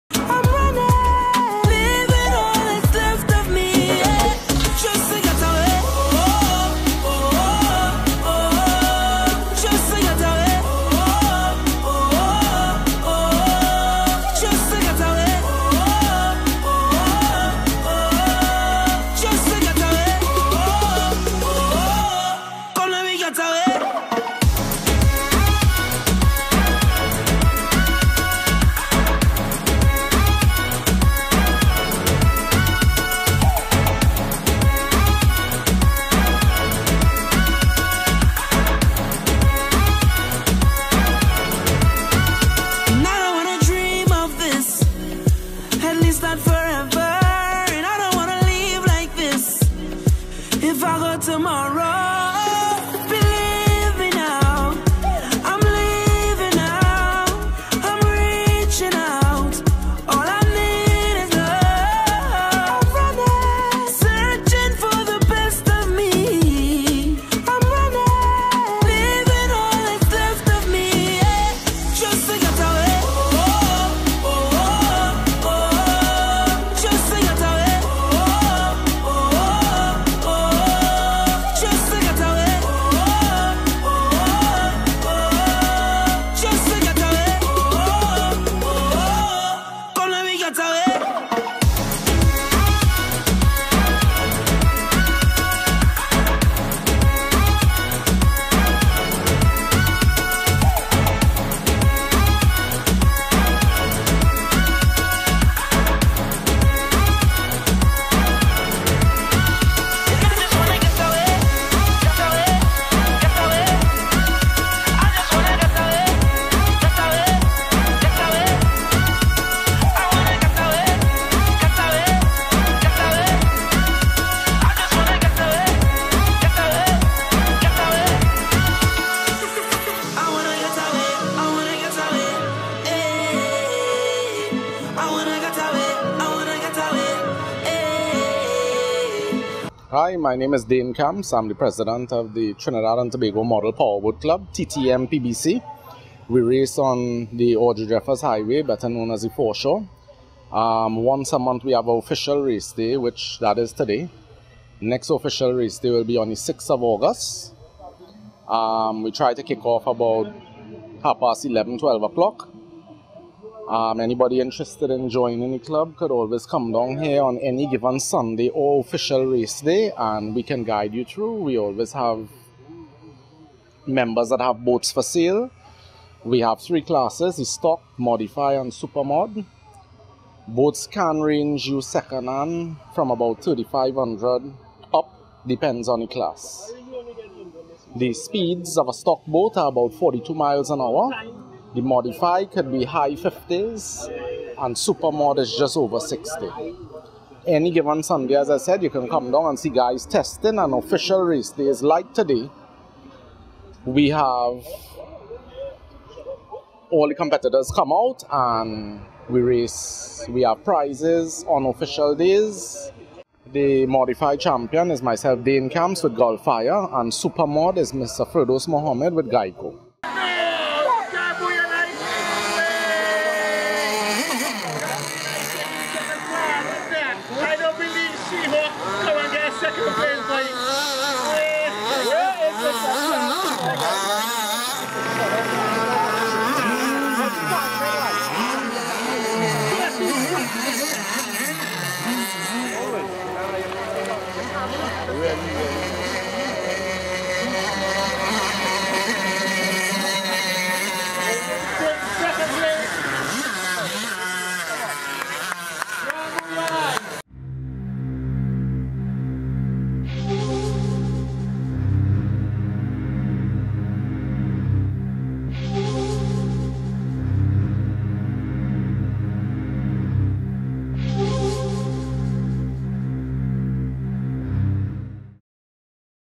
TTMPBC Model Boat Racing on the Foreshore
Dane Camps, President of the Trinidad & Tobago Model Powerboat Club, explains how to get involved in the sport.

We recommend watching KVTV on your Smart TV for the full 1080P HD experience.

Please comment, tag, like or share.